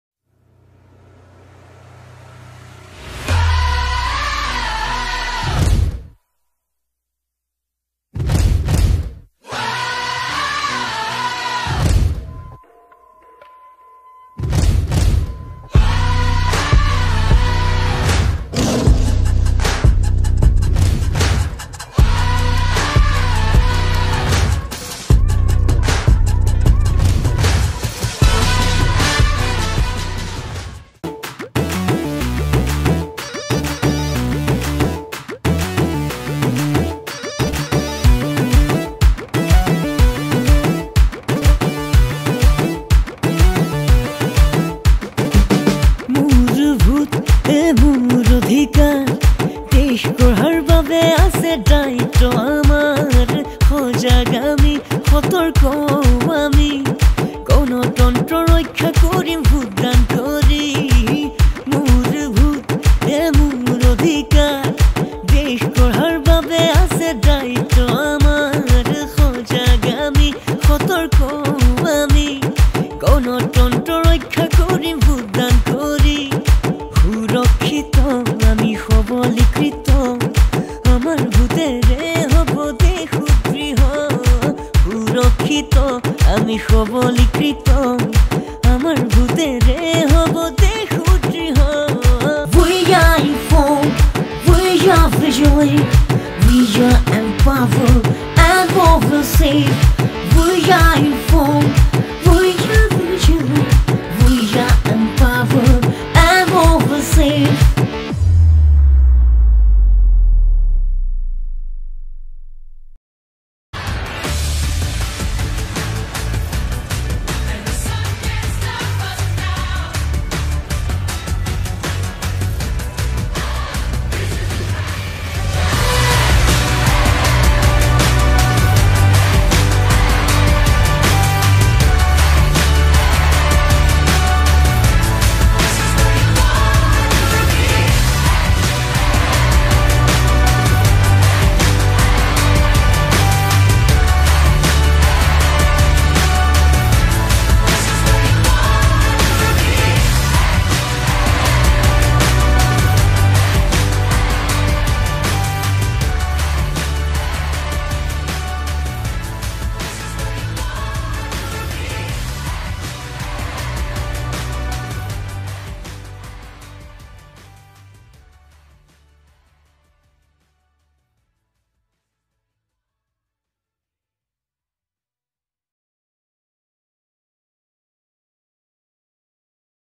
NATIONAL VOTERS DAY, ASSAMESE DANCE...
National Voters Day Special. Event Management By Aakash Sur
20 Dance Members Is This Dance Choreograph By Ajay Malakar Narengi, Organized By DistrictElectionOfficersKamrup..